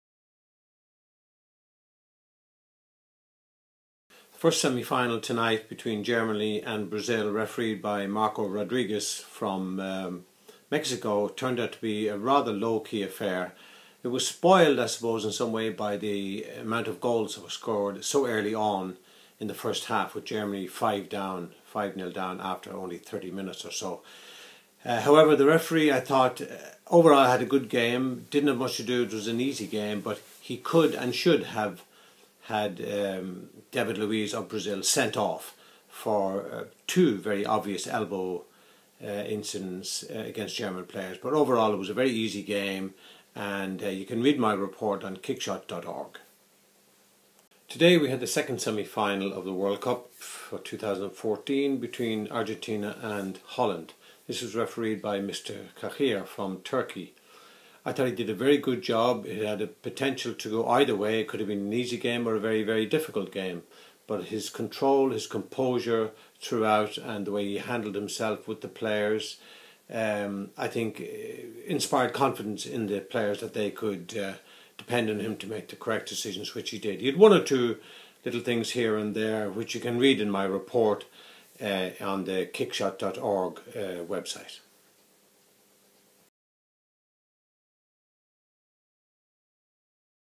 WC2014 Semi Final Days 1 and 2 Referee Analysis
July 8 and 9, 2014; Brazil v Germany (Ref: Marco Rodriguez - Mexico); Holland v Argentina (Ref: Cunyet Cahir - Turkey) For a detailed analysis, World Cup 2014, Referee Analysis by "The Hanging Judge" Dr. Errol Sweeney